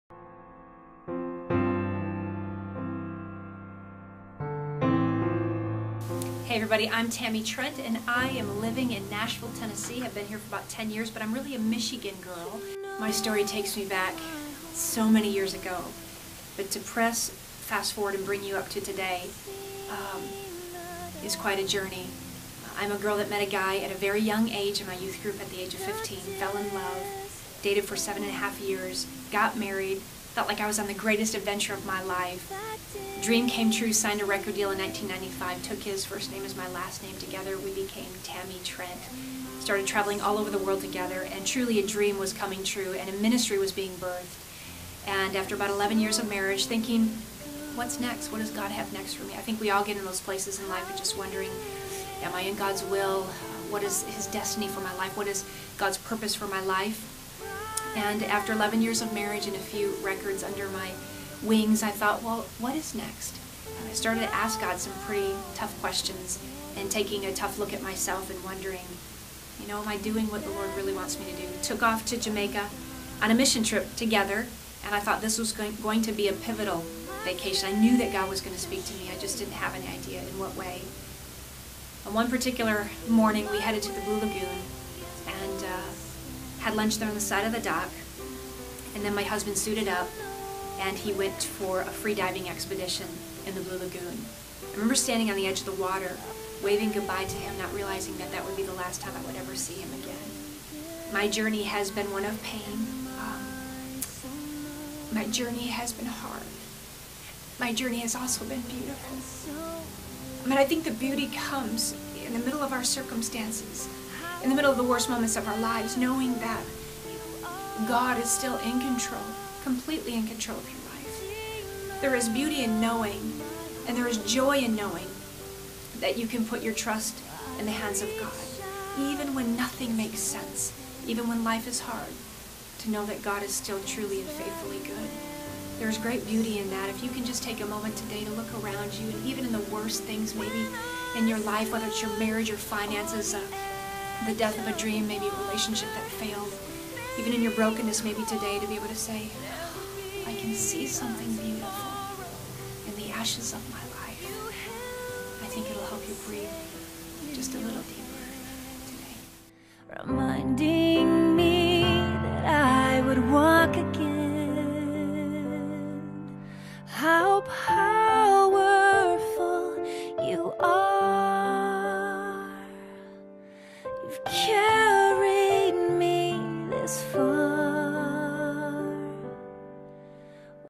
Tammy Trent: Losing the Love of Your Life*
Singer Tammy Trent shares about losing her husband in a tragic diving accident and how she can still beauty in her life despite experience tragedy. For more stories of beautiful faith, visit beautifulfaith.org; Visit tammytrent.com to find out more about Tammy's music and life.